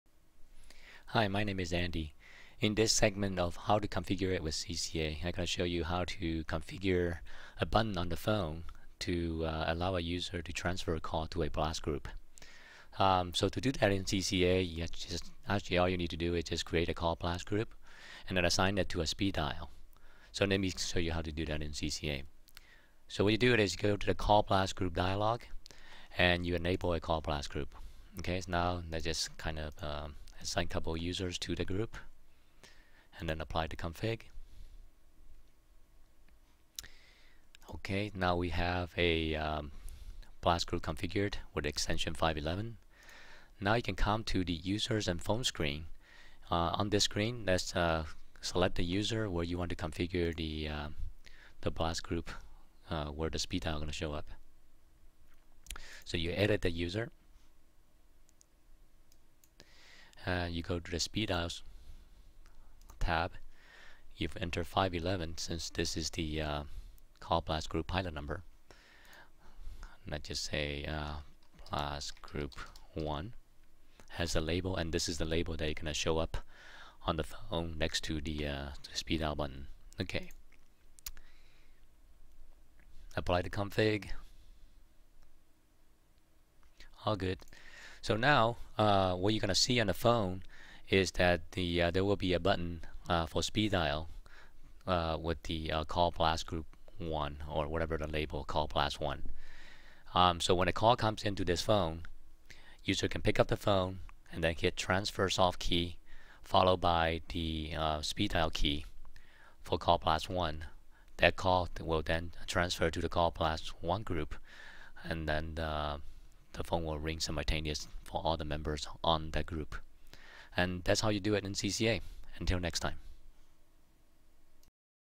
Configure a button to transfer to blast group
In this segment of "How to configure it with CCA?", I will show you how to configure a button on the phone to allow an user to transfer a call to a blast group using a speed dial.